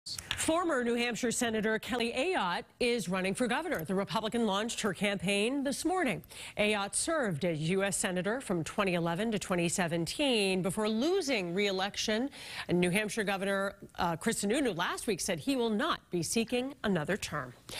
Former Senator Kelly Ayotte running for New Hampshire governor
Former New Hampshire Sen. Kelly Ayotte announced that she is running for governor in hopes of succeeding fellow Republican Chris Sununu.